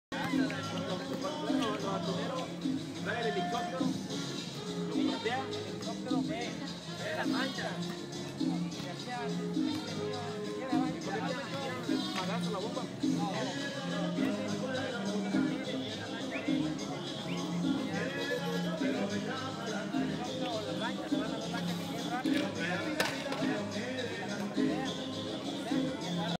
Traga fuegos trabajo honrado - Zihuatanejo
Los traga fuegos tienen un trabajo que es honrado y que a la vez es muy pesado mis respetos